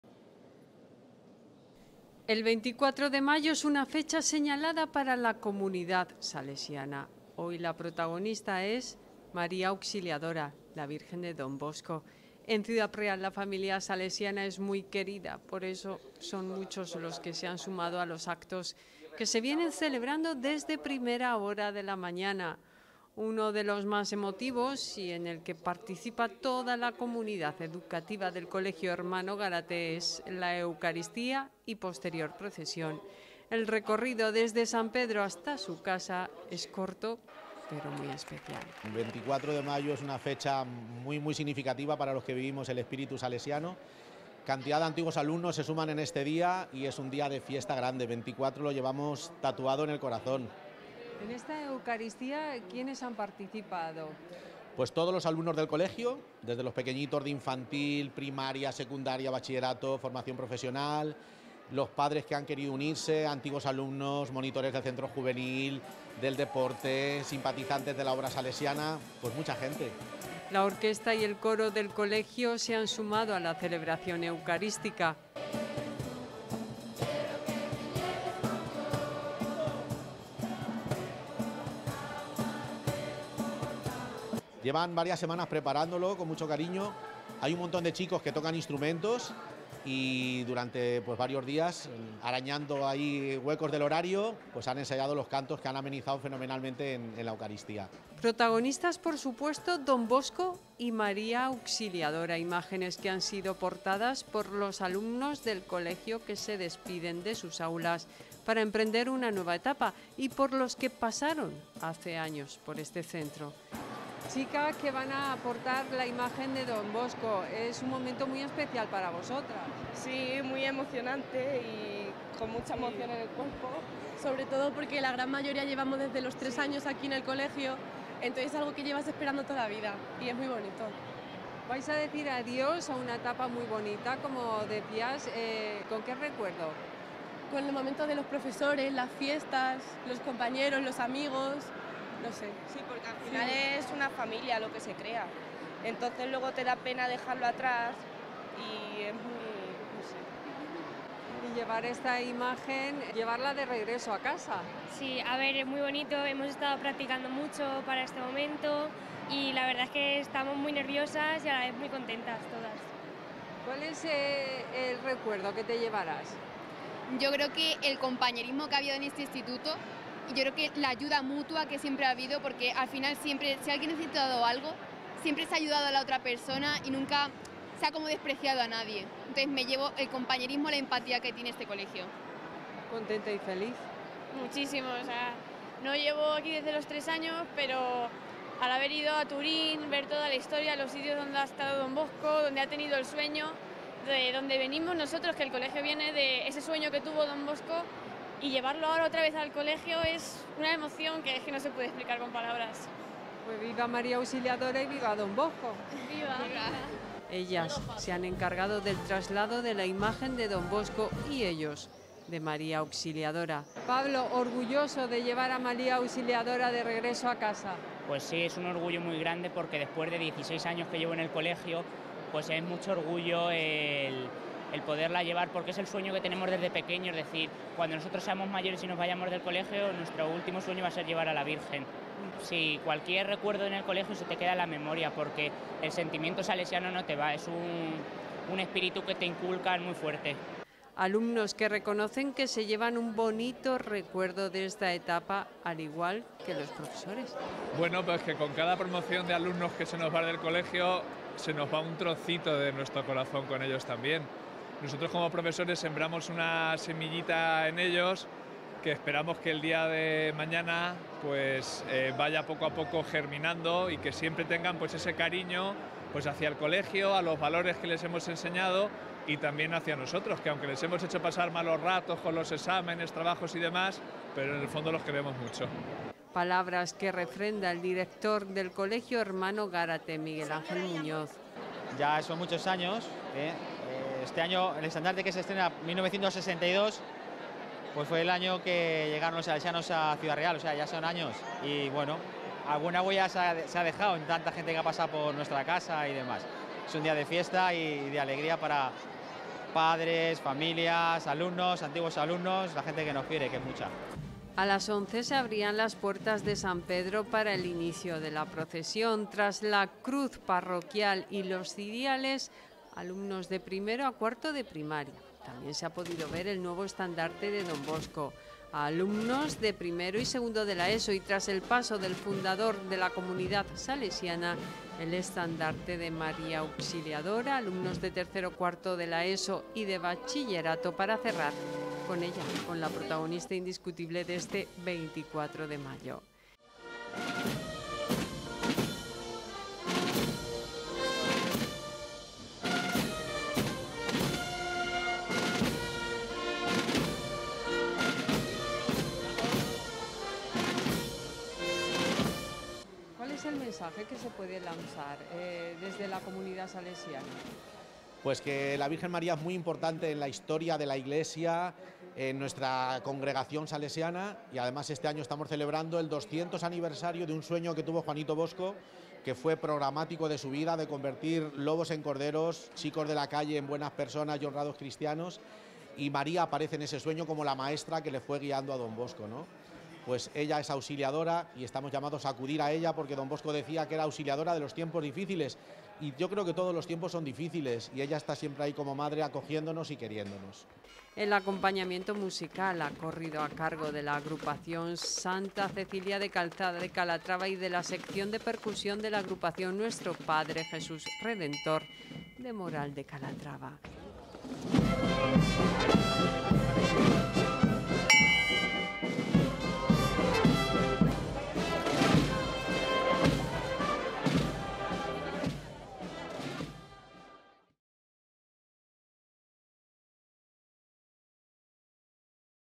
Festividad de María Auxiliadora en Ciudad Real
El 24 de mayo es una fecha señalada para la Comunidad Salesiana, hoy la protagonista es María Auxiliadora. Son muchos los actos que se están celebrando desde primera hora de la mañana en su honor, y uno de los más emotivos sin duda es, la eucaristía y posterior procesión desde la Iglesia de San Pedro hasta su casa, el Colegio Salesiano Hermano Gárate de Ciudad Real.